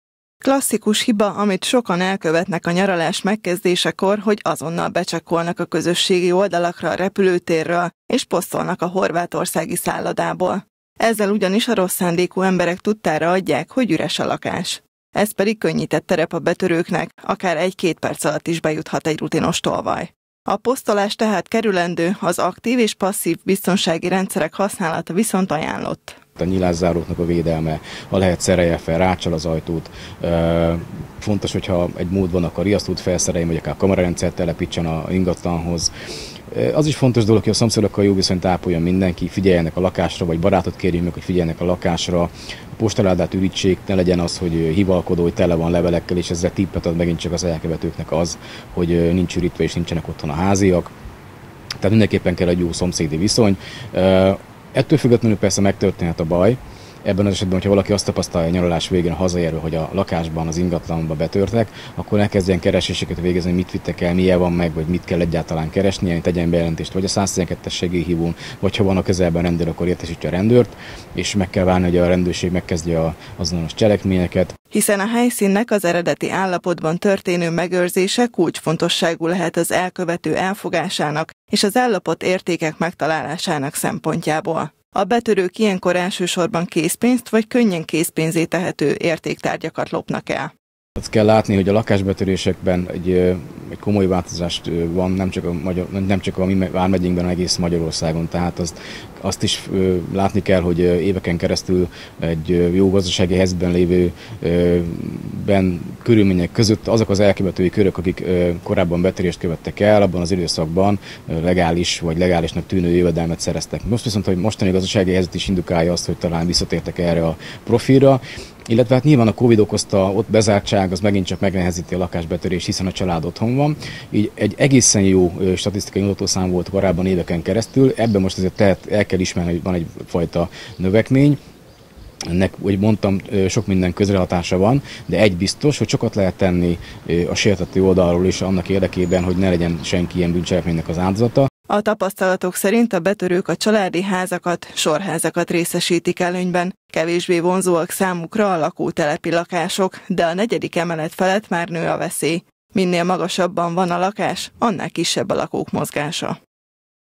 Nyári jótanácsok a rendőrségtől
Előre senki ne posztolja a közösségi oldalakon, hogy nyaralni indul – javasolja a vármegyei rendőr-főkapitányság osztályvezetője. Kovács Attila azt mondta: a bűnözők kifigyelhetik, hogy üres lesz a lakás és kipakolhatják értékeinket. Elsősorban a családi és a sorházak eshetnek így a betörők áldozatául.